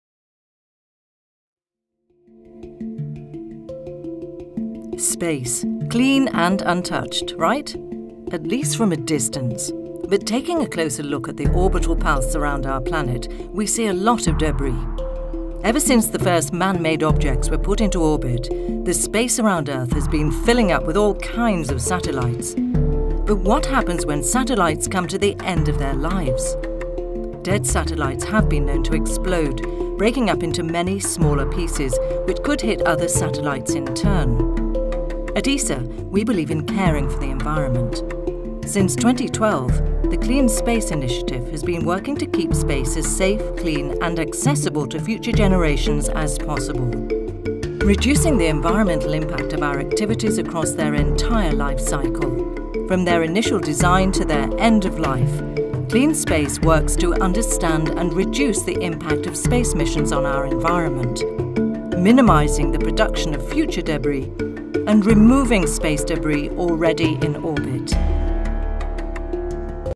Space… clean and untouched?
The ESA’s Clean Space initiative works hard to keep space safe and clean for future generations. Its three main objectives are presented in this video.